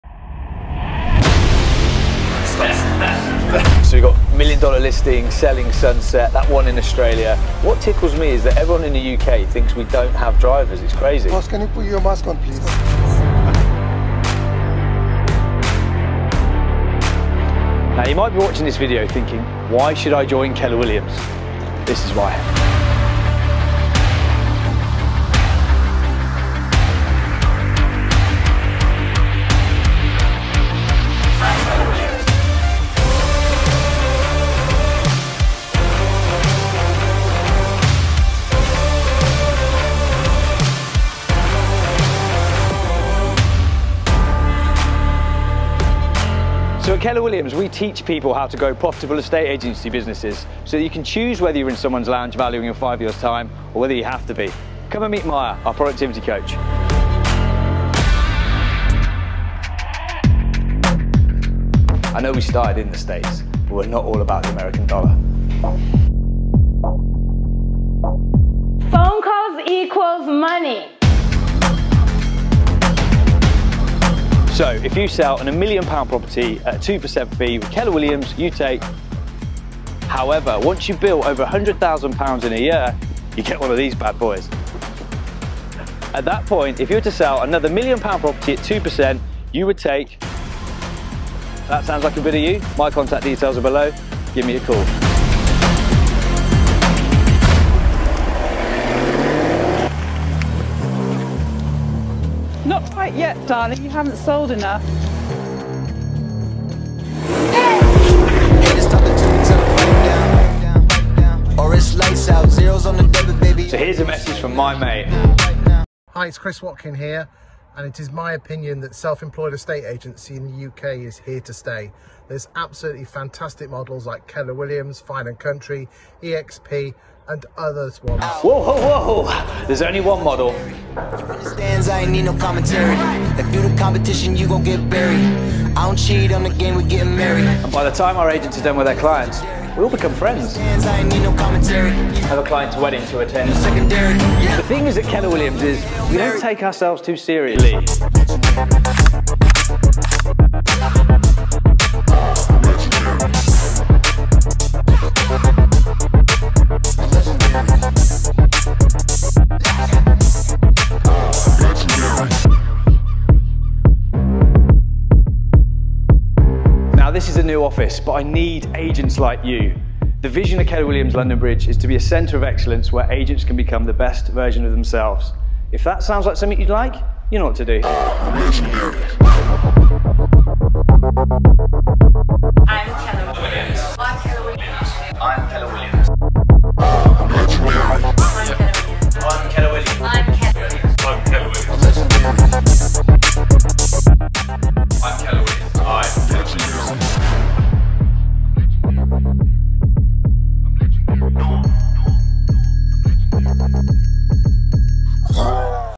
Keller Williams - what is it like being part of Keller Williams?
Keller Williams is changing estate agency in the UK by putting agents first. By being part of the worlds largest estate agent you can earn up to 98% of each sale.

Keller Williams helps estate agents go from employed, to self employed, to business ownership. The model is changing the way clients view agents.  Find out that we don't take ourselves too seriously - well kinda!

This video is a snapshot of the Keller Williams London Bridge Market Centre, showing you how you can become a business owner supported by the global leader and follow a documented formula of success. Proven in over 55 countries, Keller Williams guide you on our models and systems to ensure you keep start-up costs to a minimum and give you an unfair share of tools and technology. Join over 190,000 agents worldwide and define your own goals and objectives with a supportive team ready to help you thrive.